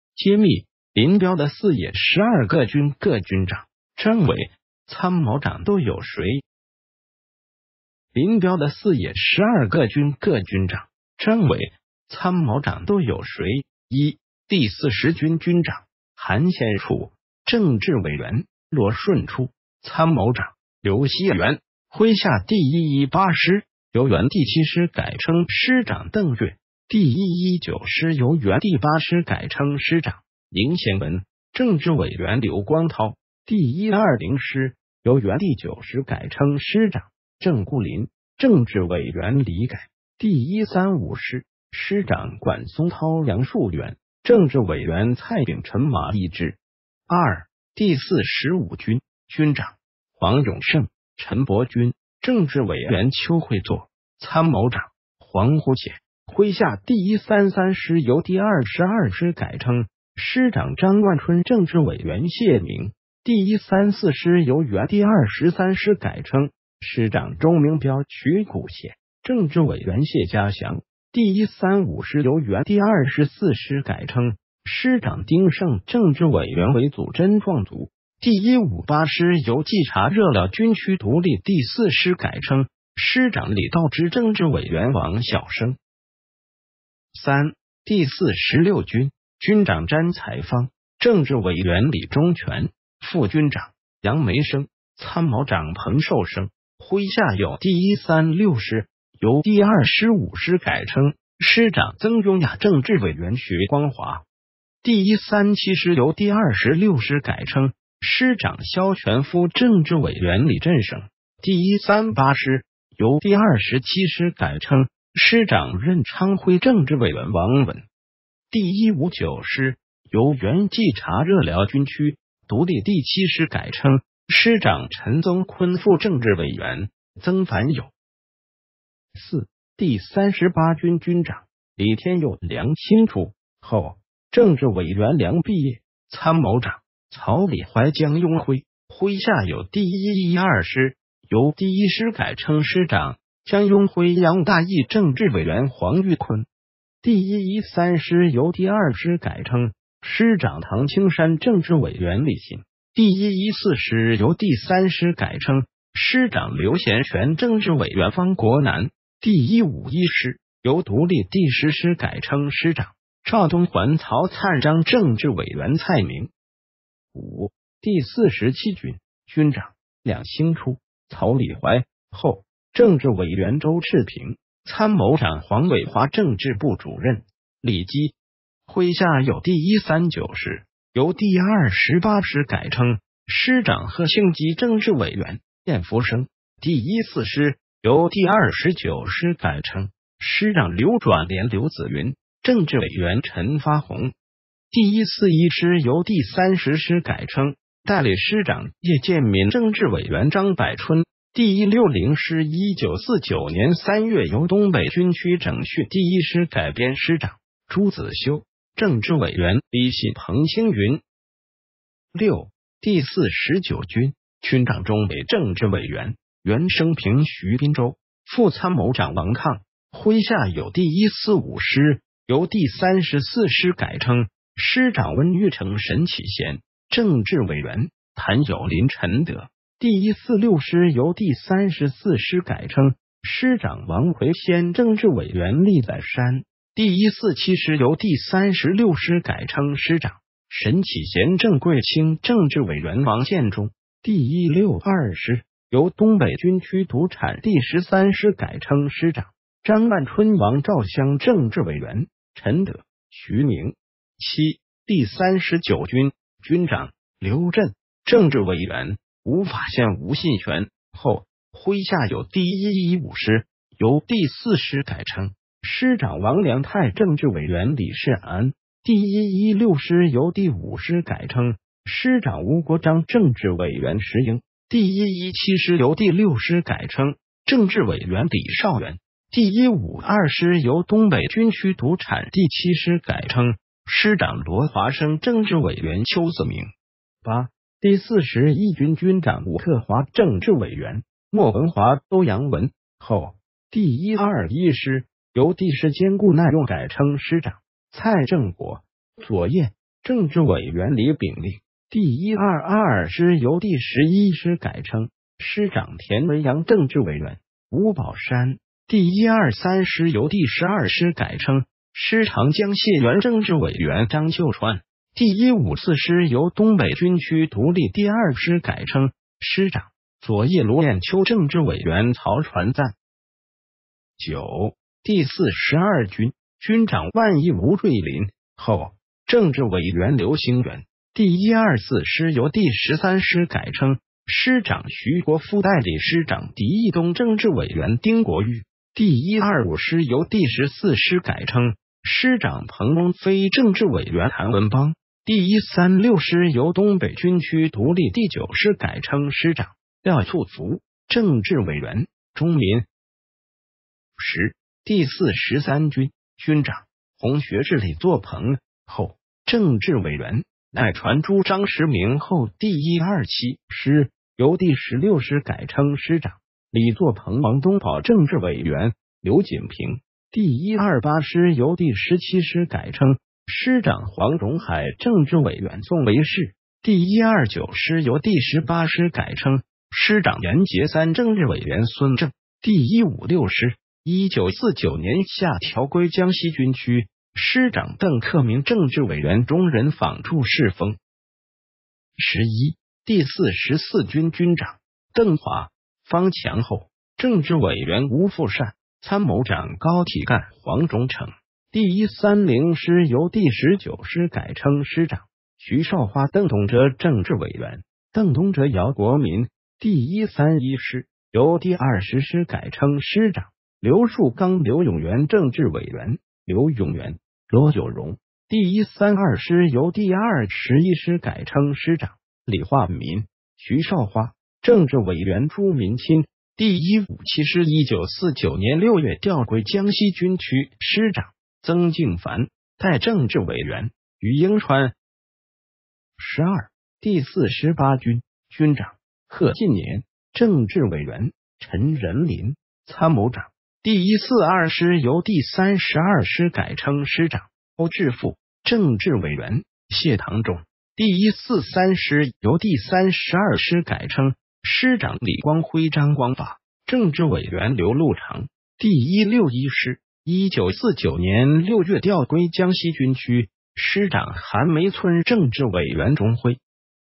揭秘：林彪的四野“十二个军”各军长，政委，参谋长都有谁？
林彪的四野“十二个军”各军长，政委，参谋长都有谁？
1，第四十军
军长 韩先楚。政治委员 罗舜初。参谋长 刘西元。麾下第一一八师（由原第七师改称）师长邓岳。第一一九师（由原第八师改称）师长 宁贤文 政治委员刘光涛。第一二○师（由原第九师改称）师长 郑固林 政治委员李改。第一三五师 师长管松涛杨树元 政治委员蔡炳臣马毅之。
2，第四十五军
军长 黄永胜 陈伯钧 政治委员邱会作。参谋长 黄鹄显。麾下第一三三师（由第二十二师改称）师长张万春政治委员谢明。第一三四师（由原第二十三师改称）师长钟明彪曲鹄显 政治委员谢家祥。第一三五师（由原第二十四师改称）师长丁盛政治委员韦祖珍（壮族）。第一五八师（由冀察热辽军区独立第四师改称）师长李道之政治委员王晓生。
3，第四十六军
军长詹才芳，政治委员李中权，副军长 杨梅生。参谋长彭寿生。麾下有第一三六师（由第二十五师改称）师长曾雍雅政治委员徐光华。第一三七师（由第二十六师改称）师长萧全夫政治委员李振声。第一三八师（由第二十七师改称）师长任昌辉政治委员王文。第一五九师（由原冀察热辽军区独立第七师改称）师长陈宗坤副政治委员 曾凡有。

4，第三十八军
军长 李天佑梁兴初（后） 政治委员梁必业。参谋长 曹里怀江拥辉。麾下有第一一二师（由第一师改称）师长 江拥辉杨大易政治委员黄玉昆。第一一三师（由第二师改称）师长唐青山政治委员李欣。第一一四师（由第三师改称）师长刘贤权政治委员方国南。第一五一师 （由独立第十师改称）师长 赵东寰曹灿章政治委员蔡明。
5，第四十七军
军长 梁兴初 曹里怀（后） 政治委员周赤萍。参谋长黄炜华政治部主任李基。麾下有第一三九师（由第二十八师改称）师长贺庆积政治委员 晏福生。第一四○师（由第二十九师改称）。师长刘转连刘子云 政治委员陈发洪。第一四一师（由第三十师改称）代理师长叶健民政治委员张百春。第一六○师（1949年3月由东北军区整训第一师改编）师长 朱子修 政治委员李信彭清云。
6，第四十九军
军长钟伟政治委员 袁升平徐斌洲。 副参谋长王亢。麾下有第一四五师（由第三十四师改称）师长温玉成沈启贤 政治委员 谭友林陈德。第一四六师（由第三十四师改称）师长王奎先政治委员栗在山。第一四七师（由第三十六师改称）师长 沈启贤郑贵卿政治委员王建中。第一六二师 （由东北军区独产第十三师改称）师长 张万春王兆相政治委员 陈德 徐明。
7，第三十九军
军长 刘震 政治委员 吴法宪吴信泉（后）麾下有第一一五师 （由第四师改称）师长王良太政治委员李世安。第一一六师（由第五师改称）师长吴国璋政治委员石瑛。第一一七师（由第六师改称）政治委员李少元。第一五二师（由东北军区独产第七师改称）师长罗华生政治委员邱子明。

8，第四十一军
军长吴克华政治委员 莫文骅欧阳文（后）。第一二一师（由第十坚固耐用改称）师长 蔡正国 左叶 政治委员李丙令。第一二二师（由第十一师改称）。师长田维扬政治委员吴保山。第一二三师（由第十二师改称）师长江燮元政治委员张秀川。第一五四师（由东北军区独立第二师改称）、师长 左叶卢燕秋政治委员曹传赞。
9，第四十二军
军长万毅吴瑞林（后） 政治委员刘兴元。第一二四师（由第十三师改称）师长徐国夫代理师长翟毅东政治委员丁国钰。第一二五师（由第十四师改称）师长彭龙飞政治委员谭文邦。第一三六师（由东北军区独立第九师改称）师长 廖促符 政治委员 钟民。
10，第四十三军
军长 洪学智李作鹏（后） 政治委员 赖传珠张池明（后）第一二七师（由第十六师改称）师长 李作鹏王东保政治委员刘锦平。第一二八师（由第十七师改称）师长黄荣海政治委员宋维栻。第一二九师（由第十八师改称）师长阎捷三政治委员孙正。第一五六师 （1949年夏调归江西军区）师长邓克明政治委员钟人仿祝世风。

11，第四十四军
军长 邓华 方强（后） 政治委员吴富善。参谋长高体乾黄忠诚 。第一三○师（由第十九师改称）师长 徐绍花邓东哲政治委员 邓东哲姚国民。第一三一师 （由第二十师改称）师长 刘述刚刘永源政治委员 刘永源 罗友荣。第一三二师（由第二十一师改称）师长 李化民 徐绍花 政治委员朱民亲。第一五七师（1949年6月调归江西军区）师长 曾敬烦 代政治委员 于英川。
12，第四十八军
军长 贺晋年 政治委员 陈仁麟。参谋长 。第一四二师（由第三十二师改称）师长 欧致富 政治委员 谢镗忠。第一四三师（由第三十二师改称）师长李光辉张光发 政治委员刘禄长。第一六一师 （1949年6月调归江西军区）师长韩梅村政治委员钟辉。